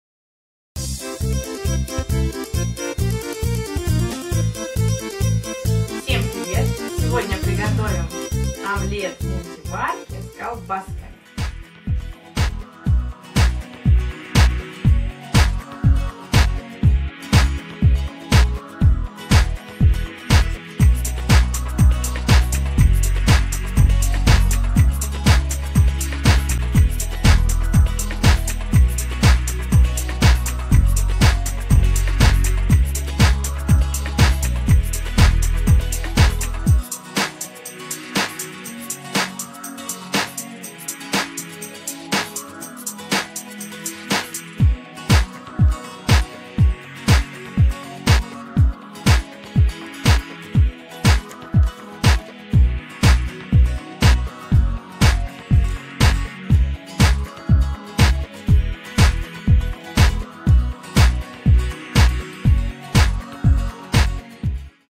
«Ленивый» омлет в мультиварке с колбасками и овощами | Рецепт Вкусного Омлета
Омлет в мультиварке рецепт и ингредиенты:
Охотничьи колбаски - 100 гр
Маринованные шампиньоны - 100 гр
Помидоры - 50 гр
Отварной картофель - 50 гр

Приготовление:
1. Разбиваем 4 яйца, взбиваем, солим
2. Вливаем в яйца молоко, взбиваем
3. Добавляем нарезанные колбаски, шампиньоны, помидоры и картофель (по желанию, все ингредиенты можно предварительно обжарить)
4. Смазываем масло чашу мультиварки, выливаем в неё получившуюся смесь
5. Ставим в мультиварку на 20 минут, включаем режим «выпечка»
6. Достаём, посыпаем зеленью

Приятного аппетита!